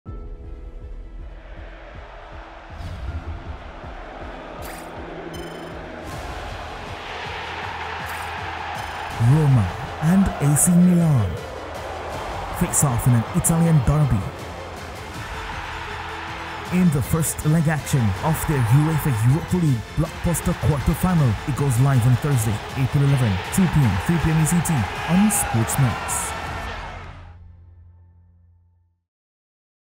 Watch the UEFA Europa League | AC Milan vs Roma | Thur. April.11, 2PM/ 3 ECT | on SportsMax and App!
Watch the UEFA Europa League | AC Milan vs Roma | Thur. April.11, 2PM/ 3 ECT | on SportsMax and SportsMax App!